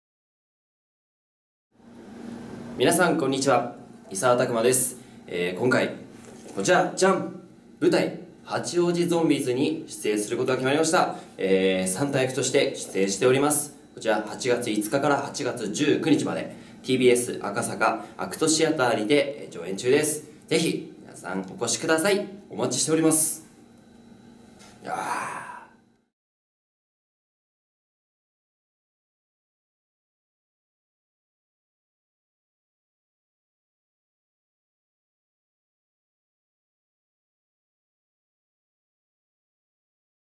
【SUNPLUS】井澤巧麻 舞台「八王子ゾンビーズ」告知
舞台「八王子ゾンビーズ」

日程・会場：
8/5(日)～8/19(日)　東京・TBS赤坂ACTシアター

脚本・演出：鈴木おさむ
主題歌：m-flo (rhythm zone / LDH MUSIC)
劇中曲：☆Taku Takahashi(m-flo, block.fm)

出演：
山下健二郎 （三代目J Soul Brothers)

久保田悠来
小澤雄太（劇団EXILE）
藤田玲
丘山晴己
高野洸
牧島輝
井澤巧麻
前田隆太朗

加藤啓
早乙女友貴
隅田美保（アジアン）

酒井敏也

RIKACO

駿河太郎